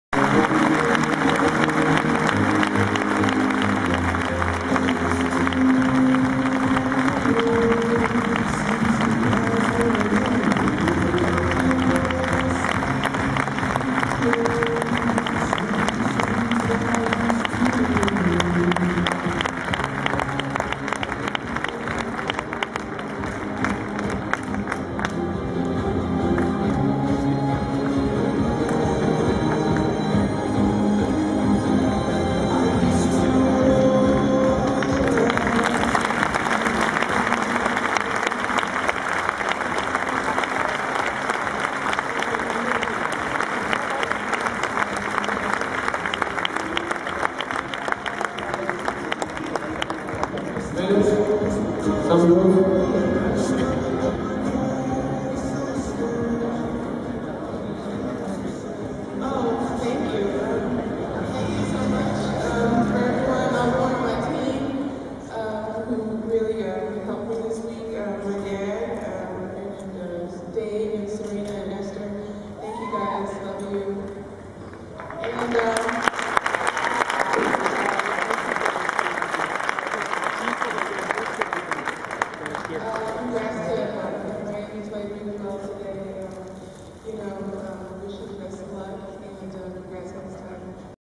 Madrid Open 2010 - Williams
Madrid Open, mayo de 2010. Premio de subcampeona para Venus Williams.